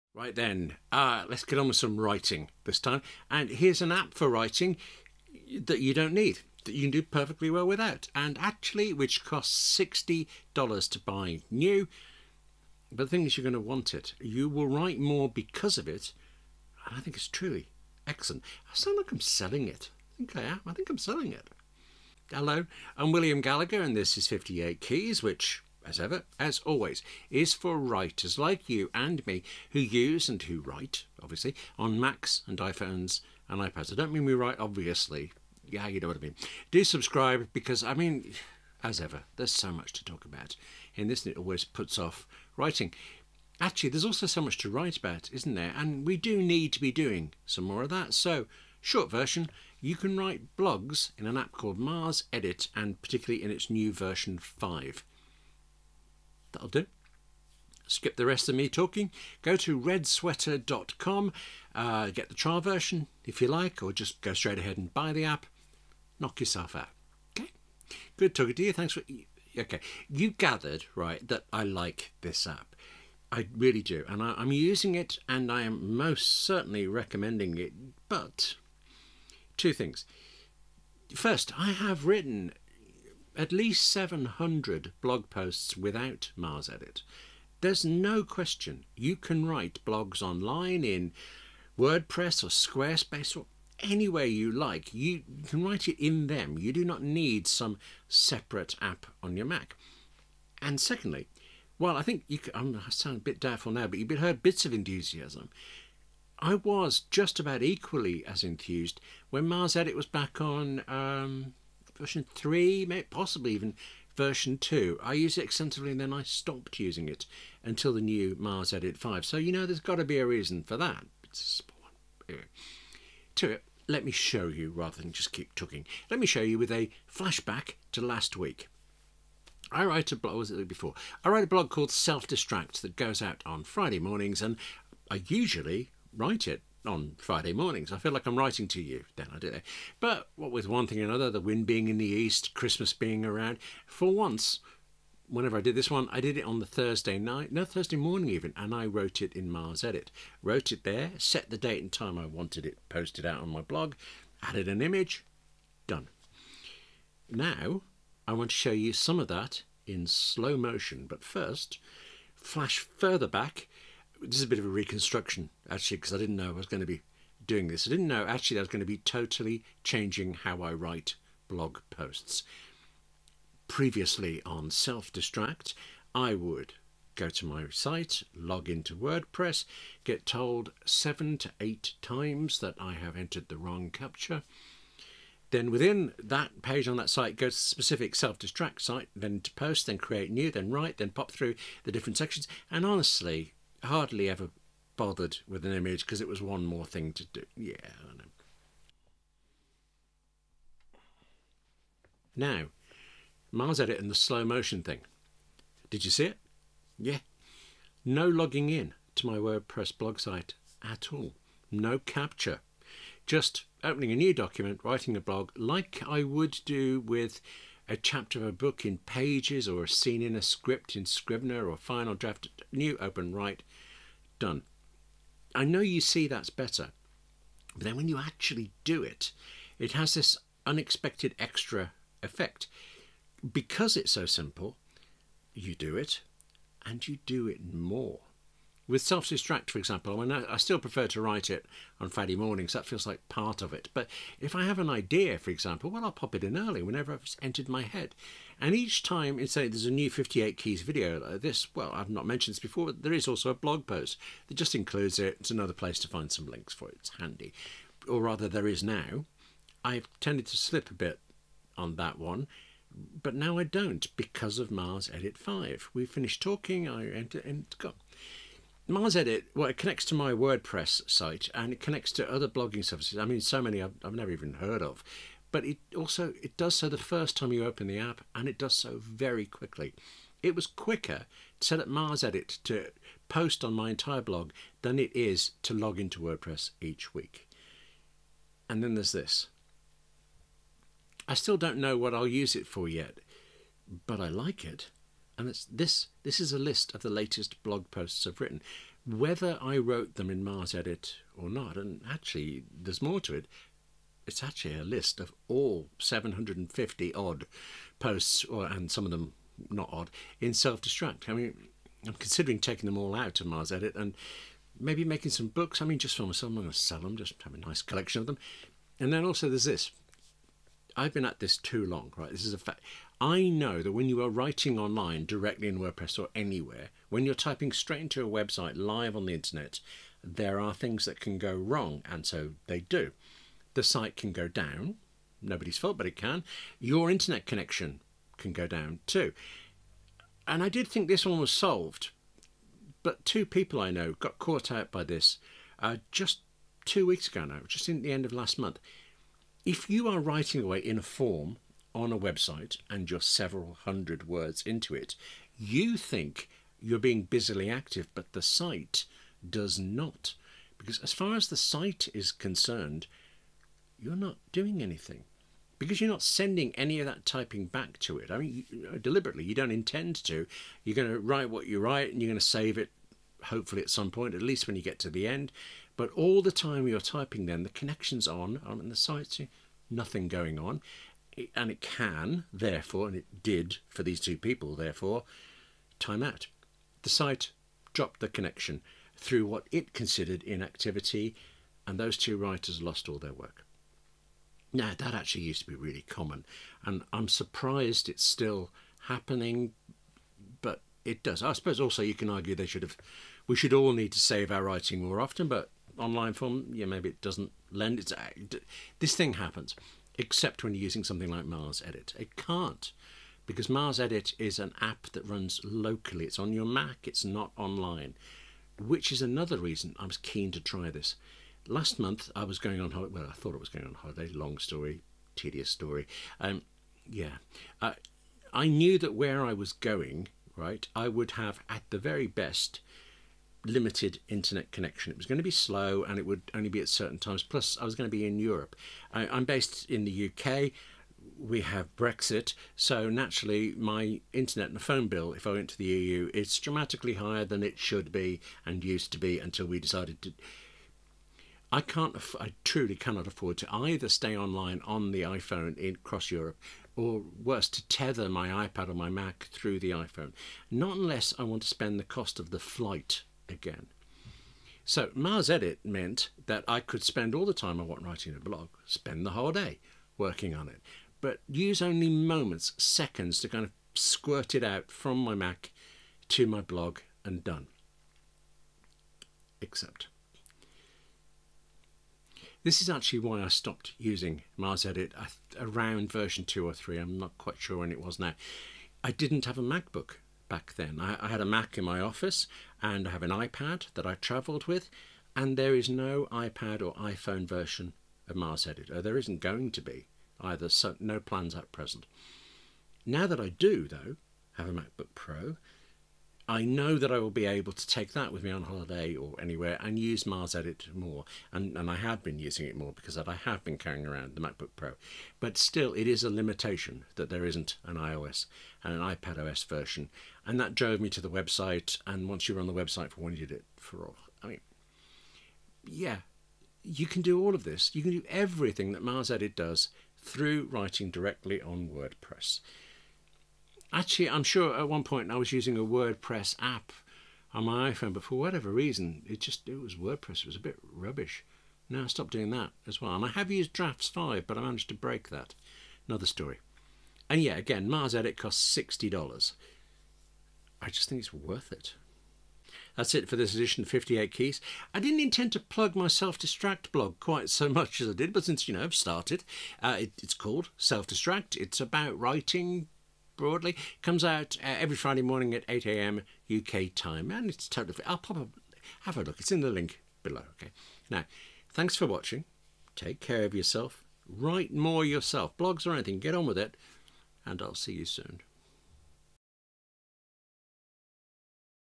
Recommended blogging app: MarsEdit 5
You do not need a blogging app, but you'll want this one and it is so good it'll make you blog more. Recommending the new MarsEdit 5 for Mac.

The Blank Screen: Productivity for Creative Writers